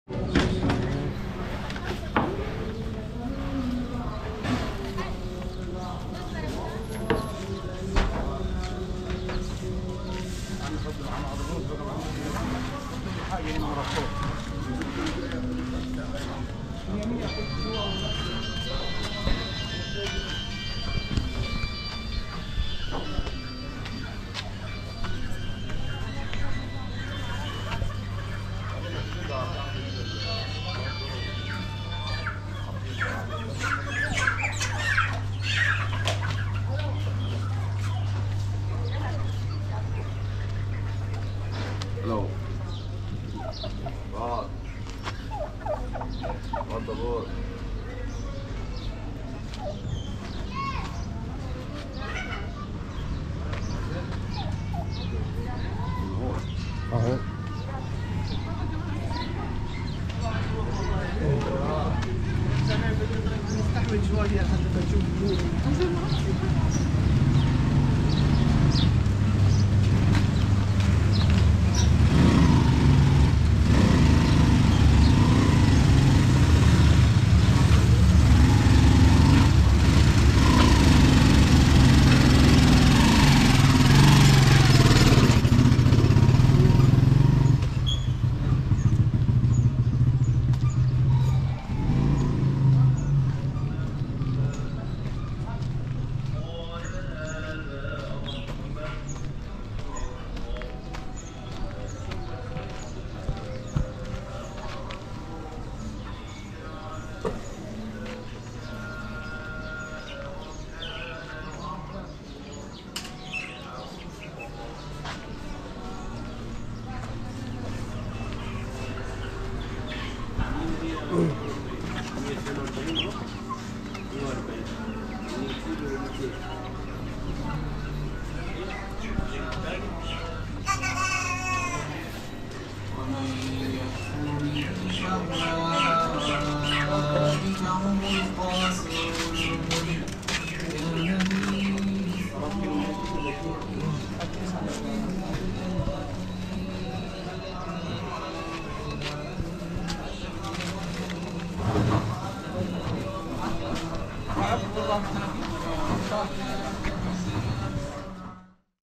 Mercado tradicional de Aswan | Walking Tour 4k
Se trata de su mercado tradicional, donde va la gente local a comprar alimentos. No hay un supermercado ni tiendas bonitas, simplemente la realidad, con sus sonidos. Si se pudiese añadir los olores la experiencia sería más realista. Simplemente, déjate llevar.